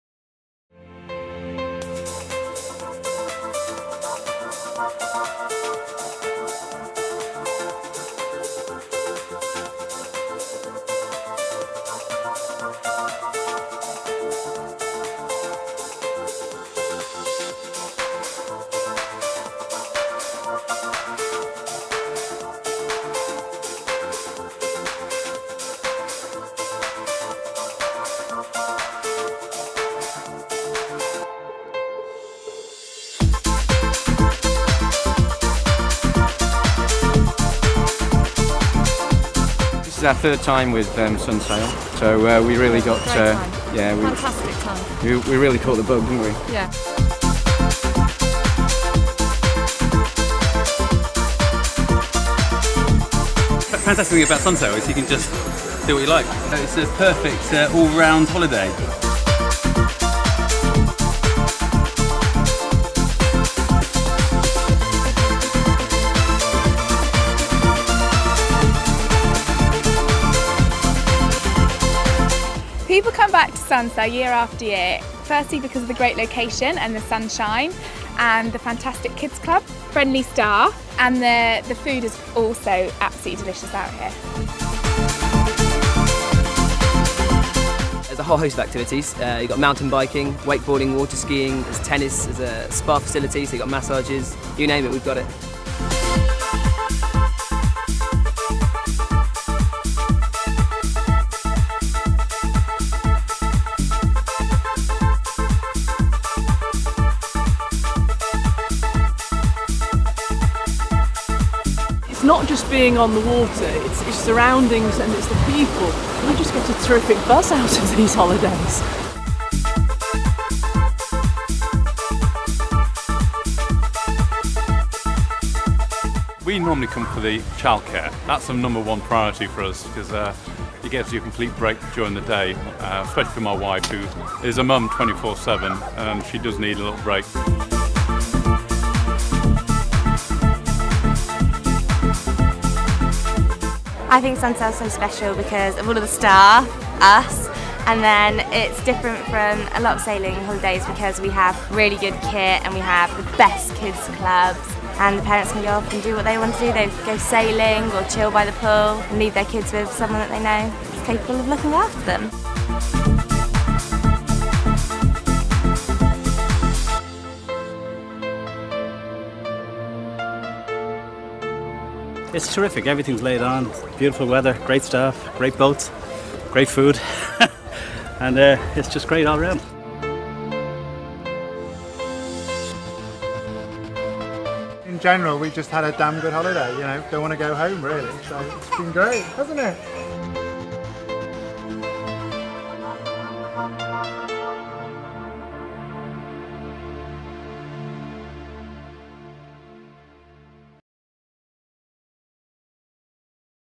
Sunsail Vacations - The experience
Experience all that Sunsail vacations offer from sailing to scuba and massages to child care, Sunsail has it all